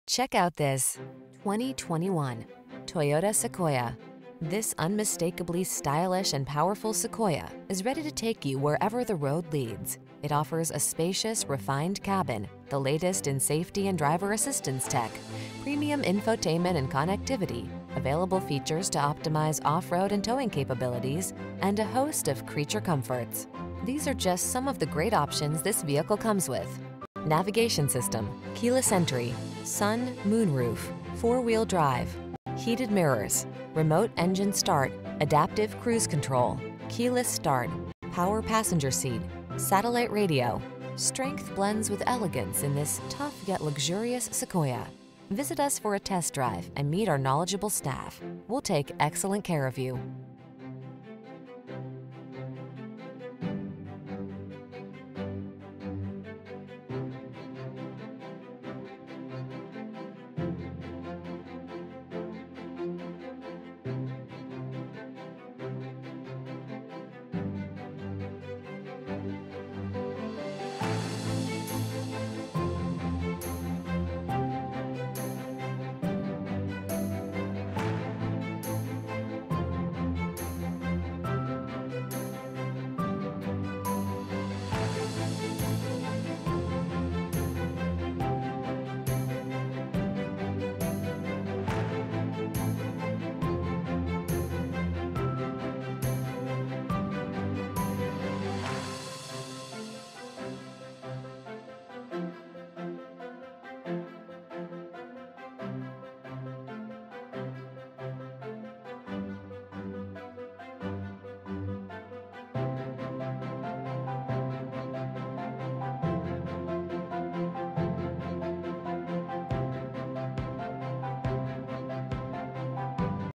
2021 Toyota Sequoia Westminster, Costa Mesa, Garden Grove, Long Beach, Huntington Beach, CA 204940
Midnight Black Metallic New 2021 Toyota Sequoia available in Huntington Beach, California at Toyota of Huntington Beach. Servicing the Costa Mesa, Garden Grove, Long Beach, Huntington Beach, CA area.